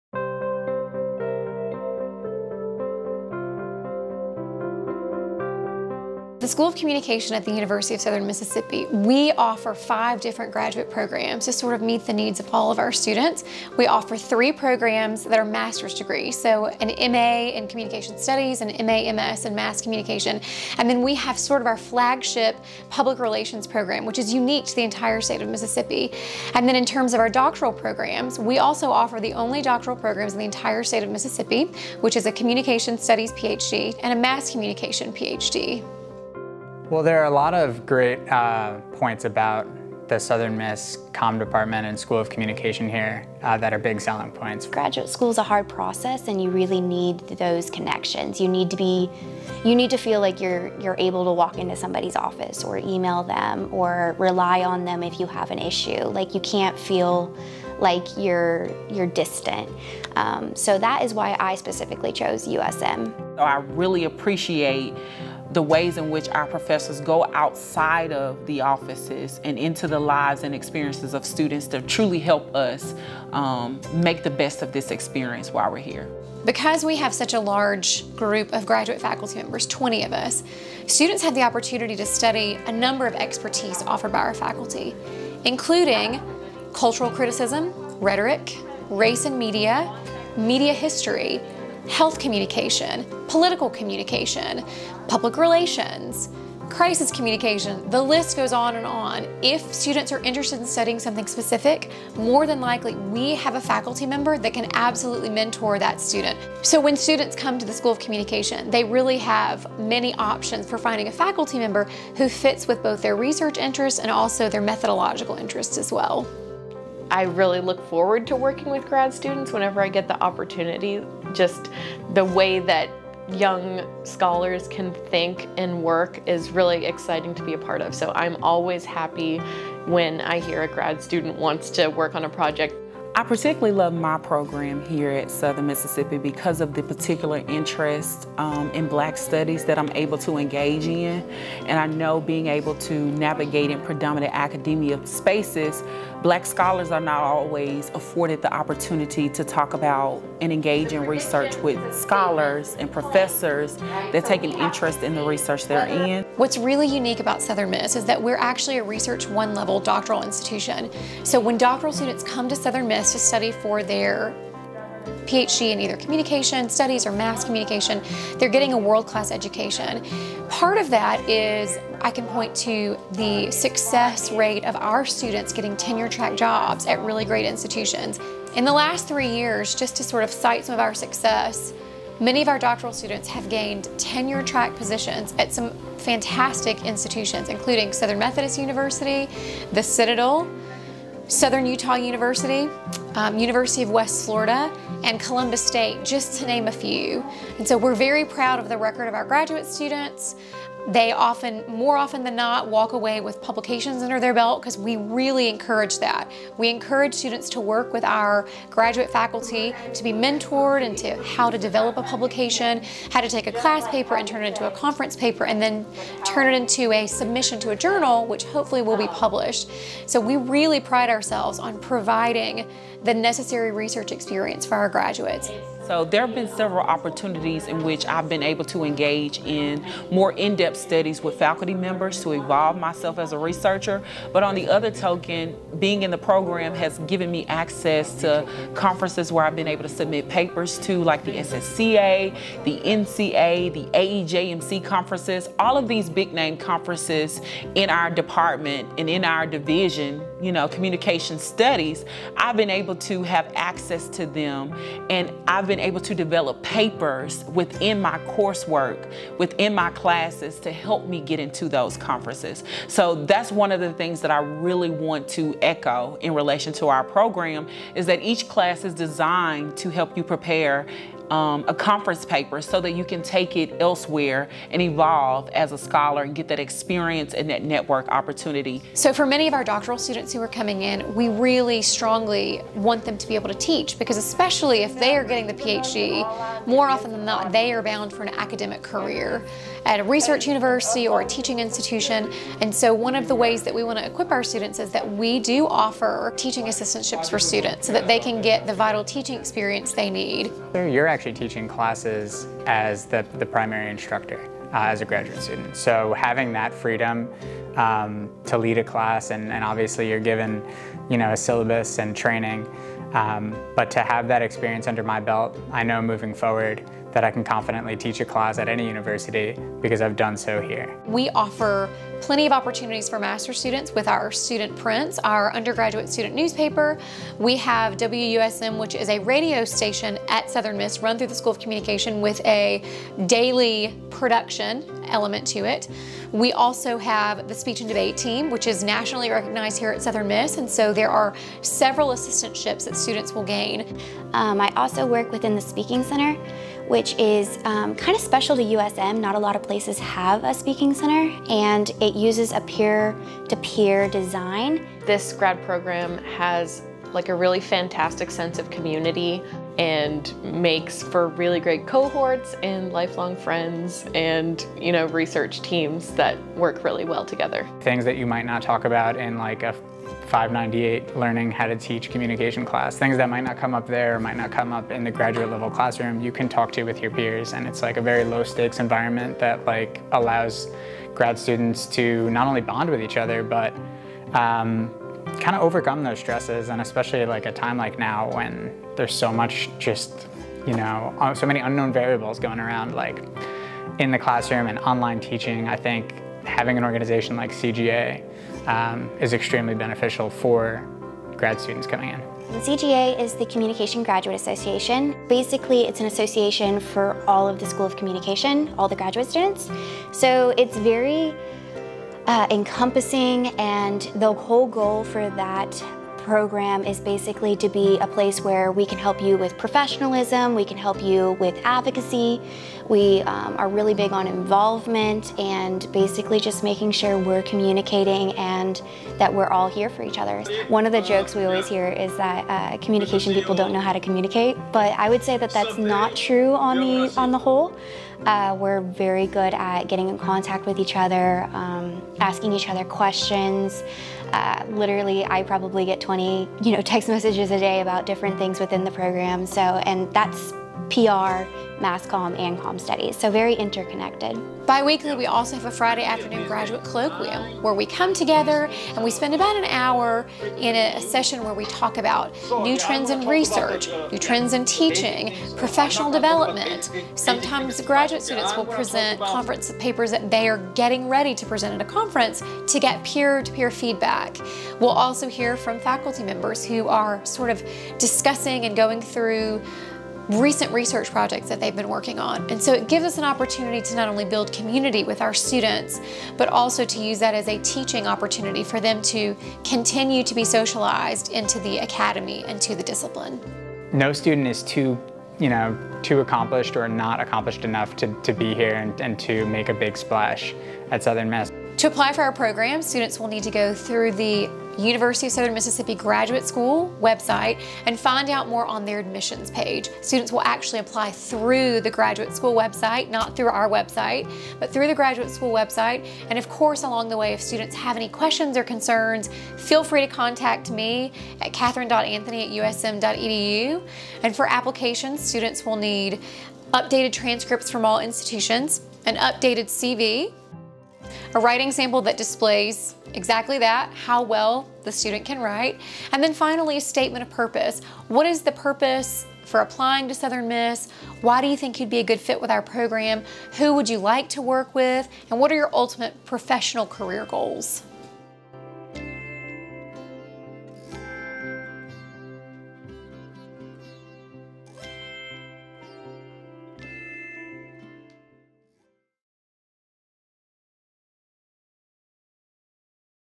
USM School of Communication Graduate Programs
Discover the graduate opportunities offered in the School of Communication at Southern Miss! From applying communication theories in scholarly research and professional projects to communication through various quantitative, qualitative, and rhetorical methodologies in multiple contexts, you will be equipped to pursue a successful career.